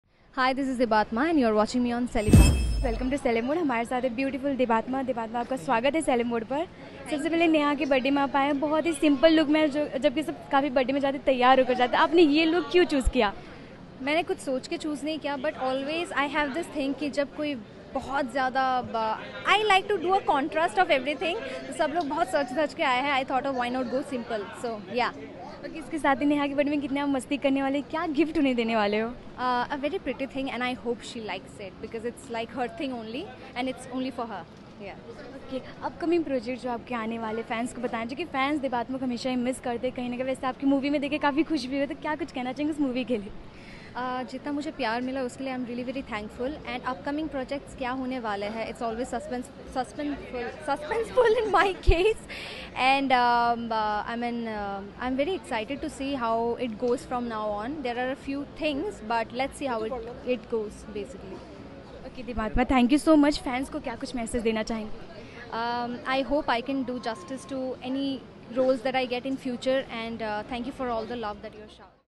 Debattama Saha Reveals Future Projects
debattamasaha who was last seen in shauryaauranokhikikahani shakhi debvir opposite karanvirsharma and shehzada with kartikaryan and mithai but today she is attending a birthday prty for a friend and wishes her the best only on celebmode